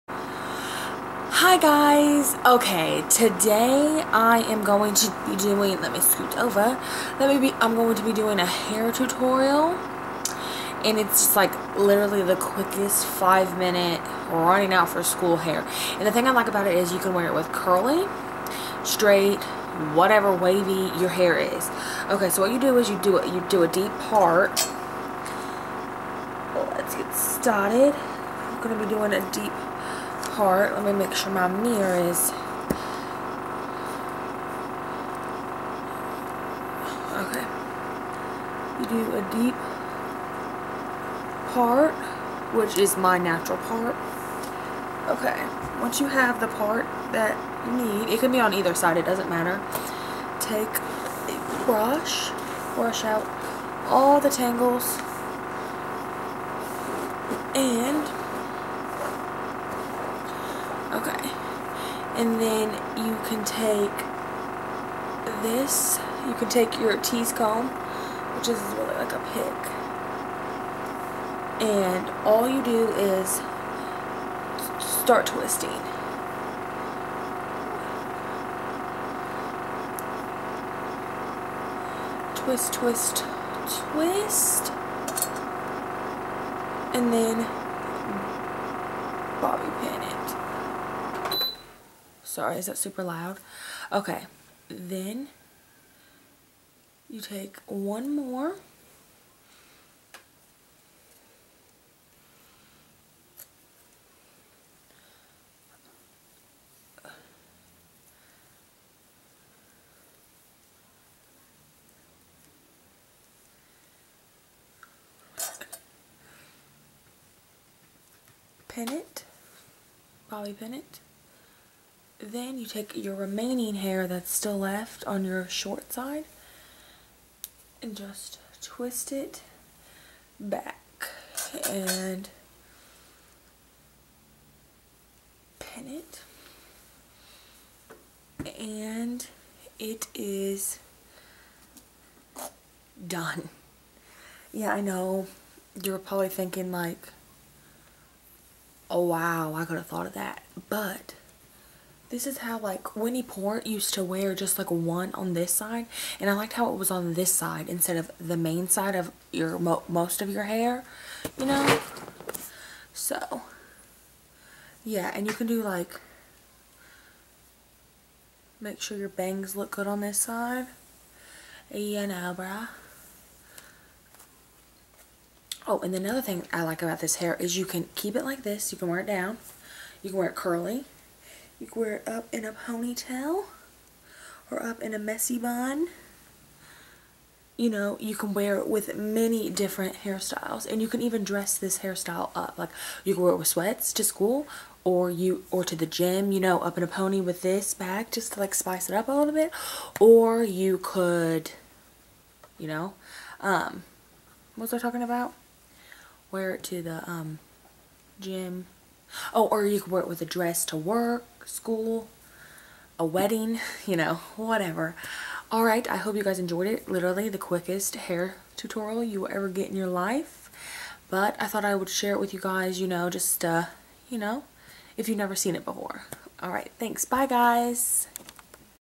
The Easiest Five Minute HairStyle For All Types of Hair!
This is the style that i like to do when im either rushing or just plan bored with my hir being straight down. I feel it gives it the extra glam that it needs to go from boring to cute. Dont forget to subscirbe!

                                                                               XOXO Makenzie